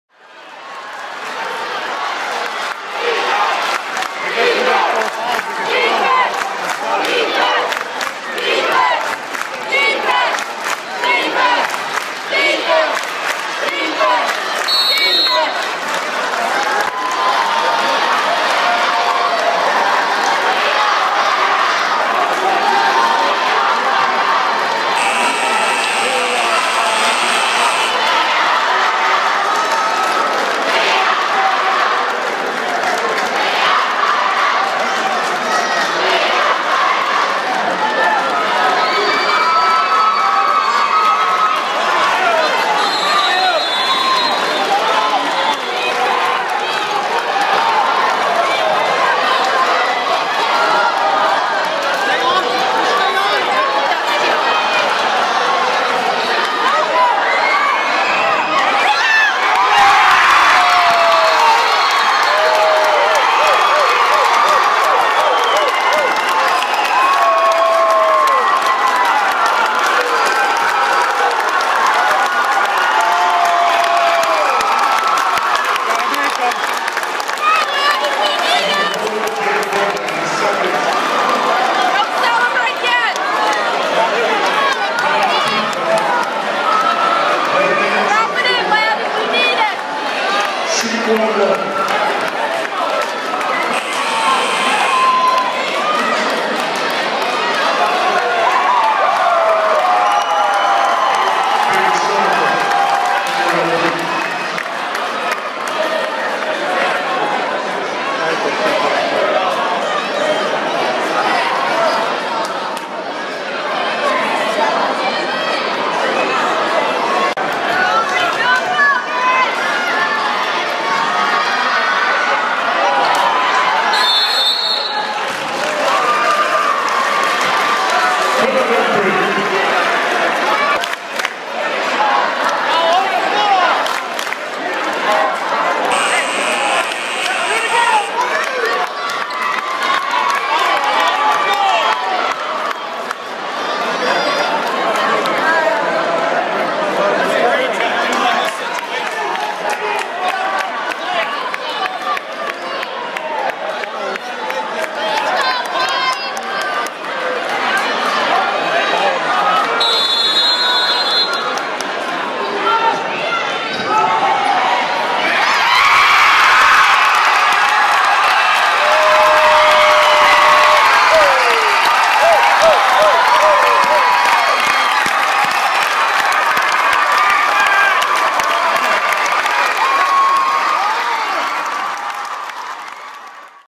Nebraska D1 Basketball Quarterfinal (2014)
End of 2014 Nebraska boys' basketball D1 quarterfinal match between Randolph Cardinals and Elm Creek Buffalos